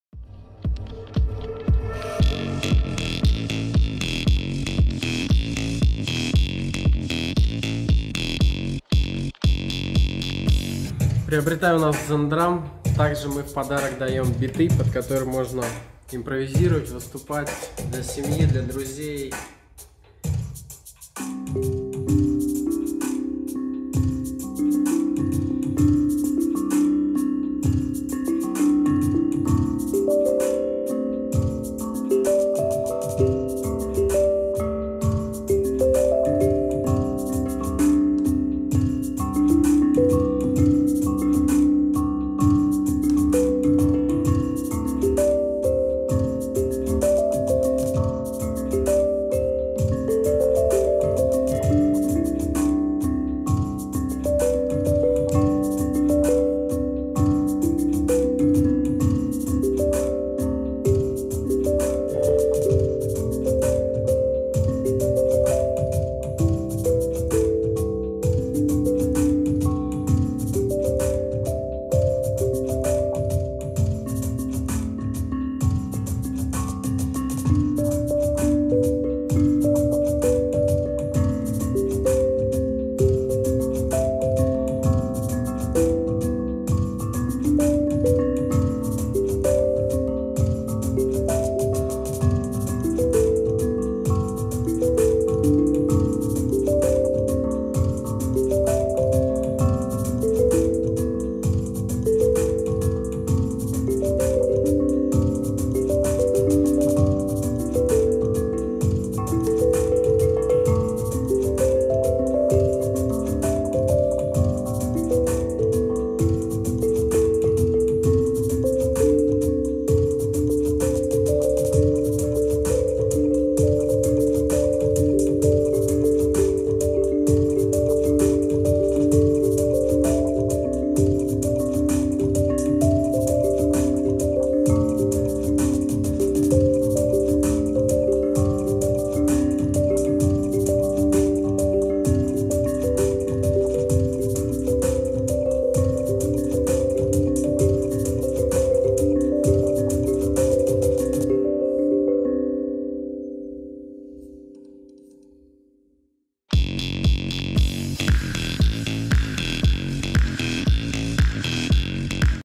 Медитируем под биты. Чарующее звучание DzenDrum'a "Душа" 12 нот. Справится даже РЕБЕНОК!
🙏🏻 Пришло время позаботиться о своей душе, вслушаться во внутреннюю пронзающую тишину и расчистить место для того, чтобы любовь заново наполнила сердце.

К чему слова в таком состоянии, когда можно общаться внутренними космосами?
Мелодия Дзен Драма "Душа"(12 нот) под бит для вас.